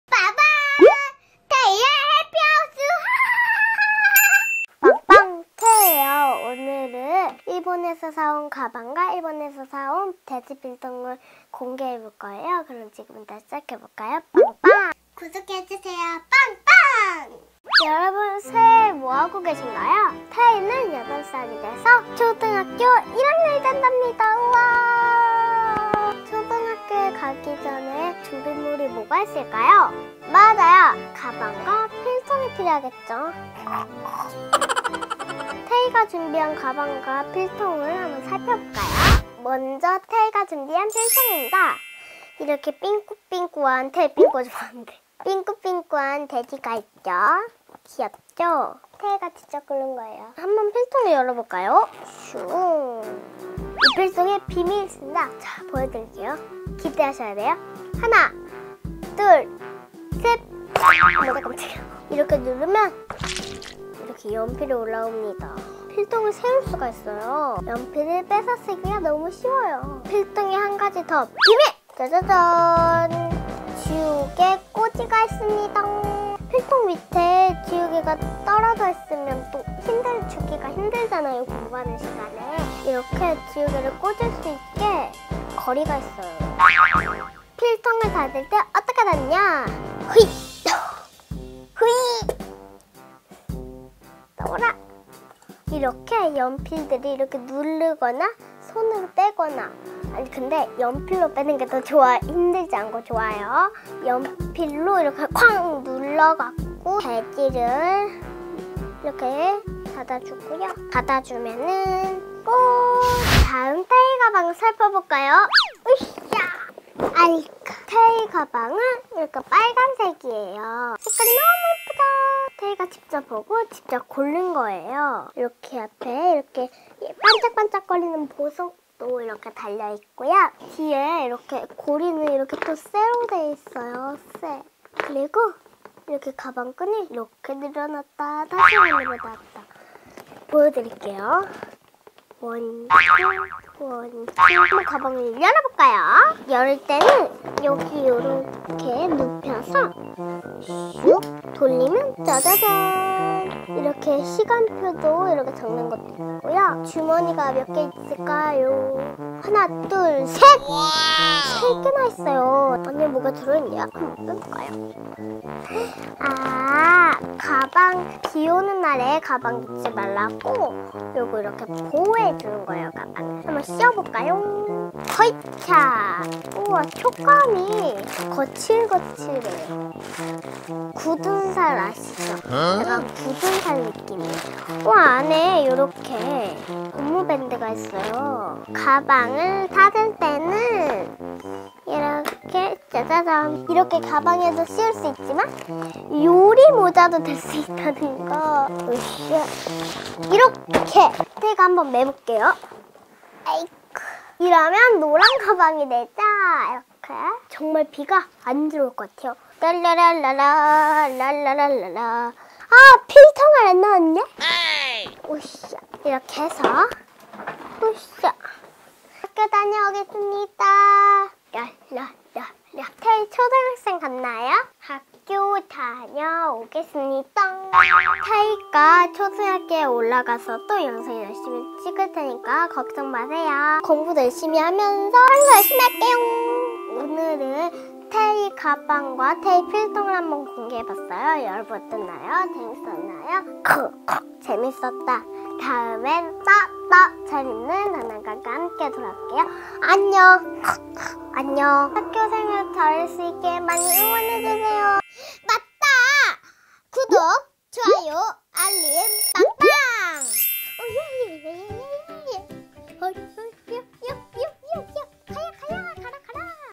[샀는데 한두번 맸나?]일본 초등학생 가방과 필통 열어 봤어요! 태희의 새학기 가방공개 란도셀ㅣ태희의 해피 하우스 '태피하'
일본 초등학생들이 쓰는 가방? 필통?
태희의 예비 초등학생 새학기 준비 란도셀 책가방 일본 필통 공개!